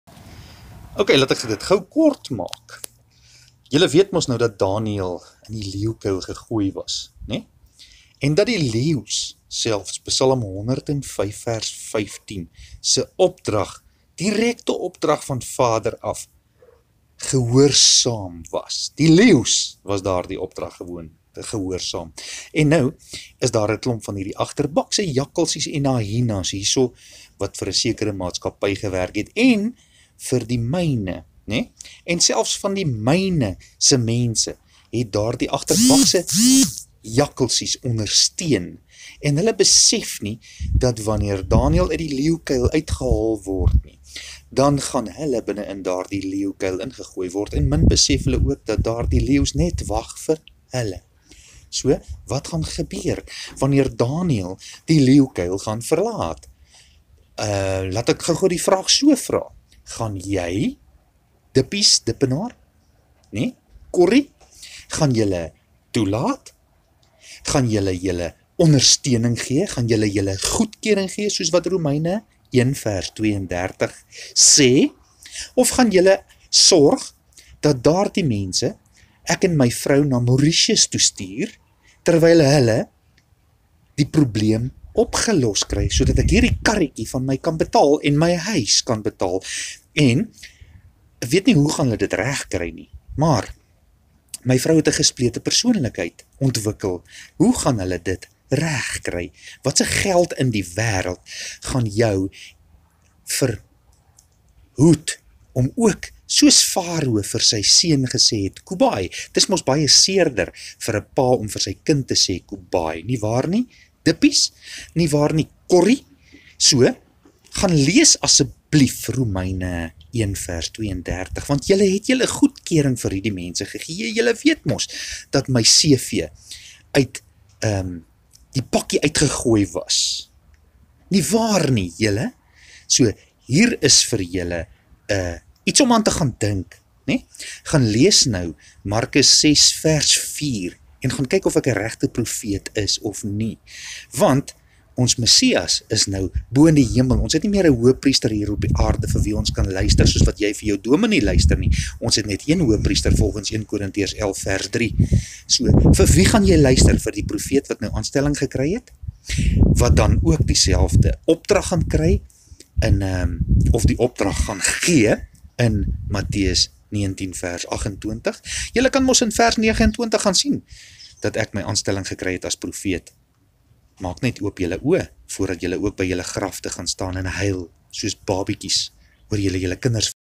Khumani mine.
Hey dames en here appels en pere, gaan daar dalk ñ Louis Pieterse binne in daardie "Klein vliegtuig" wees, wat gaan val volgens hierdie ou?

Luister gerus na hierdie Amerikaner se "prophecy"!

Tik Tok Tik Tok, my vyande dink my tyd is besig om uit te loop, ek weet my Vader se Woord sê dat ek "nooit meer" aan hulle sal dink nadat Hy met hulle afgereken het nie.

Gaan ek en Marisca dit maak? Gaan ons dalk Mauritius toe om die skade te herstel wat deur daardie agterbakse jakkalsies veroorsaak is?

Gaan my verdrukking  van Mattheus 19:29 eindig wanneer Openbaring 3:5&21 saam met Mattheus 19:28 gebeur voordat my meisiekind 17 word?

Hey, en julle het gedink my mense haat my oordat ek ñ haatlike persoon is, min het julle geweet dat dit juis "Profete" is wat deur hul eie mense gehaat word volgens twee en meer bevestigings in my Vader se Boek van die Lewe.

Kom ek haal dit vir julle aan. Lees gerus Mattheus 19:29 en kyk aan water kant van Exodus 20:5 en Exodus 20:6 se Lyn jy gestaan het in die klomp wat my vir geen rede "gehaat" het nie.

En dan vra ek jou om te gaan kyk na YaHusHa MassiaH se woorde in Markus 6:4 En YaHusHa sê vir hulle: ñ Profeet is nie ongeëerd nie, behalwe in sy vaderland en onder sy familie en in sy huis.

Hy Tobie Beukes, Kobus Botha, as ek julle kan aanraai, laat weet a.s.b die mense wat gewoontlik saam met Louis vlieg. Sê vir hulle hulle moet eers na hierdie ander persoon se profesie luister voordat hulle saam met Louitjie in ñ klein vliegtuigie klim.

Ek dink hierdie hofsaak waar hulle buite hul jurisdiction gaan beweeg het om my opgesluit te kry oor ek die "Waarheid" praat op sosiale media het dalk te vêr gegaan. En soos hulle teen die tyd weet, is ek volgens Mark 6:4 beslis een van Psalm 105:15 se "uitverkore" profete, en daardie "hofsaak" van 28 Mei 2021 het teen Psalm 105:15 se direkte opdrag gestaan. Gaan ons iets groots sien gebeur voor ons oë?

Wel, as my Vader ernstig was in Psalm 105:15 en Sy seun het in Markus 6:4 uitgewys hoe daardie "profete" uitgeken kan word, en jy het nie geluister nie, dan moet jy seker maar voel.

Kathu Praatsaam, Kathu Gesels, Kathu Verkope, Postmasburg Sê Jou Sê, sit nader, en ek dink my boetie van Amerika se myn waarvan hy praat se sinkgate mag dalk voor of op 28 Junie gebeur.

Tik Tok boys and girls. Tik Tok. Wanneer dink julle gaan daardie musiek CD's se profesie vir my begin? Natuurlik wanneer Daniel uit die Leeukuil uitkom en die agterbakse jakkalsies se verdrukking gaan begin.

Het hulle al gedink watse skokgolwe so Klein vliegtuig kan veroorsaak wanneer hy val?

Kom ek wys vir julle een van daardie golwe uit! Rom 1:32 sê, as jy jou goedkeuring gee aan die ou in die vliegtuig, dan sal jy dieselfde straf kry as hy"!

So, dink Dippies Dippenaar dat hy die golf sal vryspring teen die tyd wanneer die byl val? Of gaan hy eers sorg dat reg en geregtigheid geskied voordat die byl val en hy kry dalk trane in sy oë omdat hy nie geluister het na ñ ware profeet soos YaHusHa MassiaH vir ons ware profete uitwys in Mar 6:4 nie?

Hey oompie, voordat ek my vrou gaan verloor of op straat gaan beland, sal jy weet of ek een van Psalm 105:15 se mense was wat onder jou "godheid" deurgeloop het en jy sal of jou seun of jou dogter verloor deur my Vader se hand indien jy nie optree teen die agterbakse jakkalsies wat veroorsaak het dat ek in hierdie leeukuil gegooi is nie. So, gaan jy betyds vir Joe Lourens van Kathu uit die Leeukuil kry voordat jy deel is van die klomp wat met my plekke gaan ruil hier in die Leeukuil, jy weet mos van my CV wat uitgegooi was uit die kortlys uit nie waar nie Dippies?

Kom ons kyk of Moses van Handelinge 3:22 se profesie in vervulling gebring sal word voordat Faro se seun van hom af weg geruk gaan word.

Al wat Daniel hier vannuit die put kan sê tussen die leeus, sterkte boys, want hierdie leeus van Scarface is erg uitgehonger, hulls soek iets on te kou maar self's hulle het geweet van Psalm 105:15 se opdrag.

What you gonna do when my Dads gonna come for you? Gaan jy nog ñ bietjie slaap en nog ñ bietjie toelaat dat Joe in die Leeukuil bly nadat jy nou weet dat jou stem ook kant kies?

As jy tog maar die boodskap geseel het sou jy nie deel gewees het van die klomp wat vir die honger leeus gevoer gaan word wanneer die byl val nie, maar nou sit jy een giggel soos een van Scarface se Hyenas en min weet jy dat jy deel sal hê aan Rom 1:32 se strafgerig wat kom net soos die tsunami wat eers terugtrek om als te ontbloot voordat dit terugkeer met mag en krag.